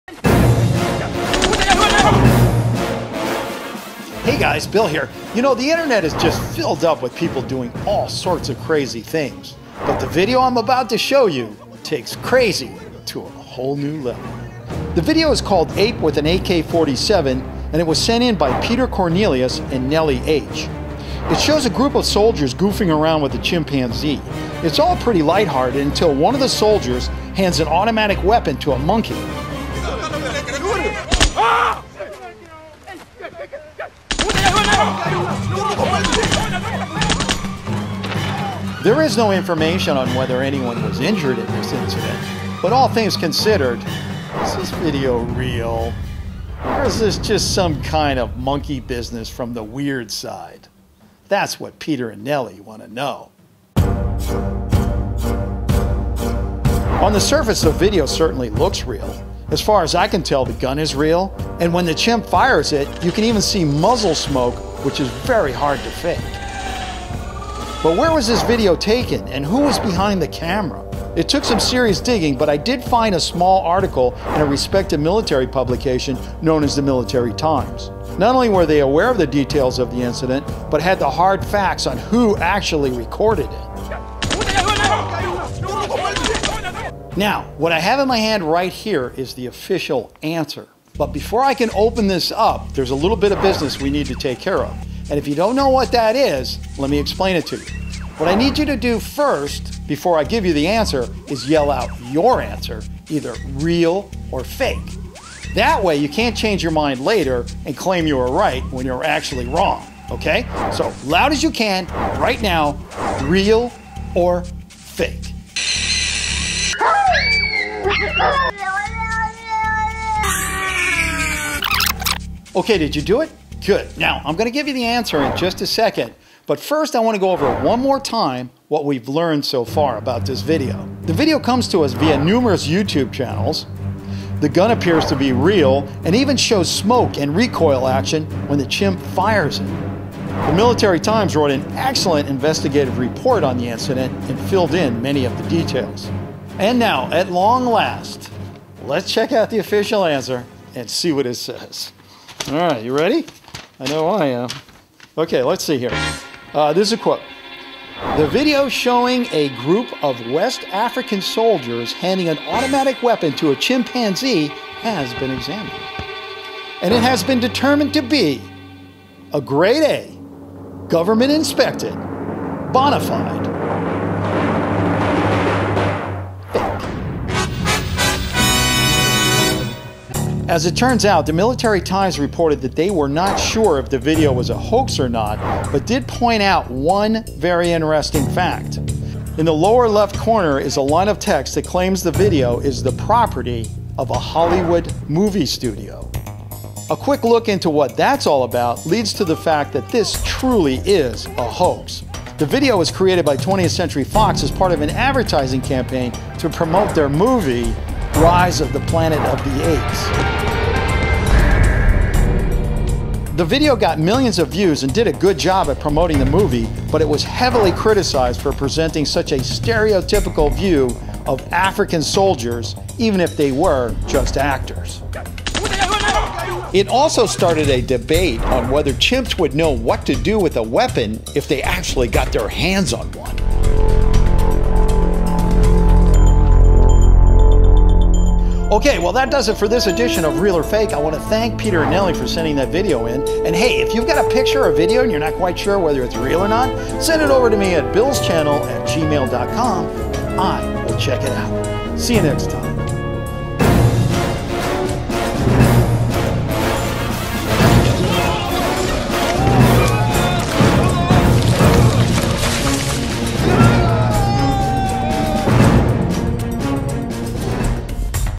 CHIMP with WEAPON - real or fake?
The internet is filled with people doing all sorts of crazy things, but in the video I am about to show you - it’s about as crazy as it can gets.

The video  was sent in by Peter Cornelis and Nelly H.

It shows  a group of soldiers goofing around with a chimpanze. It’s all pretty light hearted until one of the soldiers hands an automatic weapon to the Monkey… and then, you guessed it, the monkey starts shooting weapon.

There is no information on whether anyone was hurt in this incident, but is this video real? Or, is this just some what internet monkey business from the weird side. That’s what Peter and Nelly want to know.

On the surface, the video certainly looks real. As far as I can tell, the gun is real… and when the chimp fires it, you can even see mussle smoke which would be hard to fake.

But where was this video taken, and who was behind the camera?

It took some serious digging, but I did find a small article in a respected military publication - known as the Military Times.  Not only were they aware of the details of the incident, but had the hard facts on who actually recorded it.

Now what I have in my hand here is the official answer about this video, But before I open this up, there is a little business we need to take care… and if you don’t know what that is… let me explain it to you.

what you need to do is yell out your answer first, either real or fake… Ok That way, you can’t change you mind later and try to claim you were right when you were wrong.

So ok… ready… and go!

Alright, did you do it. Good. Hopefully you were with your friends or family and didn’t wake anybody up.

Im going to give you the official answer in just a second, but first, let;s go over what we have learned so far.

The video comes to us via numerous youtube channels…

The gun appears to be real and even shows smoke and recoil action when the chimp fires the gun.

The Military Times wrote an excellent investigative report on the incident.

And now, lets see what the official answer is… and I quote.

The video showing a group of west african soldiers handing an automatic weapon to a chimpanzee has been examined… and determined to be… a fake…

As it turns out, the military times reported they were not sure if the video was a hoax or not, but did point out one very interesting fact.

In the lower left corner is a line of text that claims the video is the property of  research department.

A quick look into what that is all about, leads to the fact that this is truly a hoax. The video was created by 2  as part of an advertising campaign to promote a movie their movie

The video got millions of views and did a good job at promoting the movie, but it was heavily criticized for presenting such a stereotypical view of African Soldiers- even if they were just actors.

It also started a debate on whether chimps would know what to do with an weapon if they got their hands on one.

Ok, that’s it for this edition of real or fake. I want to thank Peter and Nelly for sending in this video. I’m going to leave a link to the original video in the description box - And hey, if you have a picture or video that you aren’t sure if it is real or not, send it over to me at